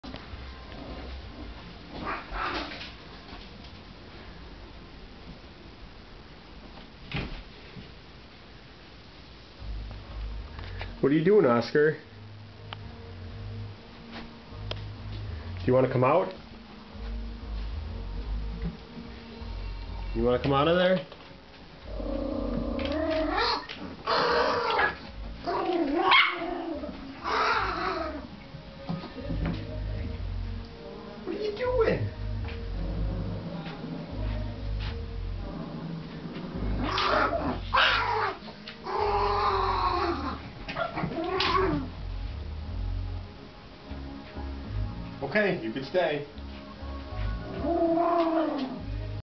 Oscar the Grouch!
Our little yorkie, Oscar, loves hiding out...this time he chose a garbage can full of fabric remnants. Our other yorkie, Gracie, just watches all of the action.